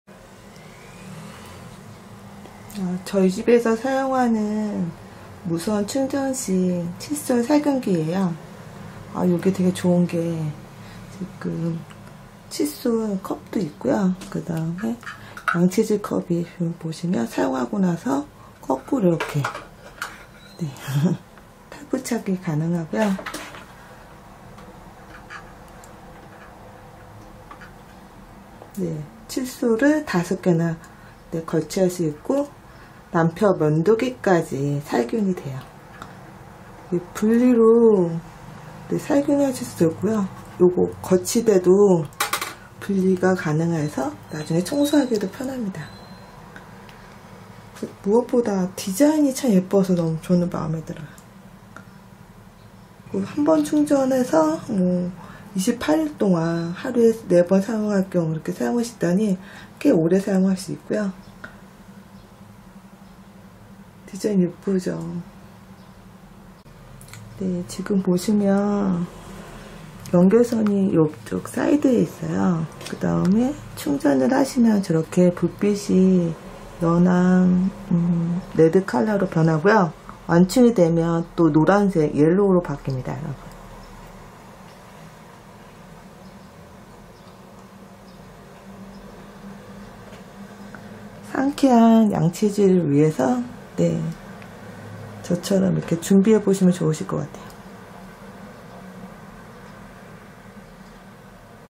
무선 칫솔살균기 면도기 동시살균 되는
어디서나 설치 가능한 충전식

유토렉스 칫솔5개에 면도기까지

동시살균이 되다니 가정용칫솔살균기 으로 딱이네요.